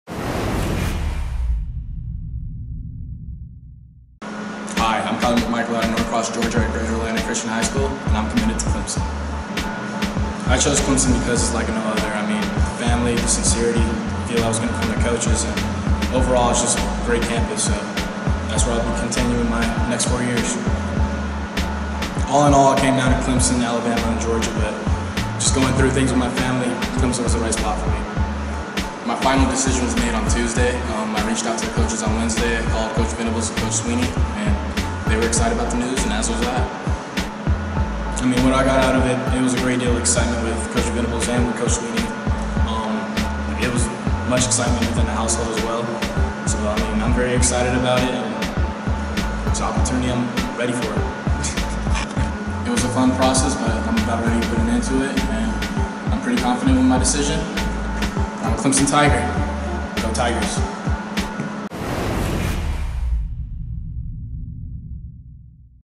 Kyler McMichael Commits to Clemson
Rivals 100 ATH Kyler McMichael breaks down his commitment to the Clemson Tigers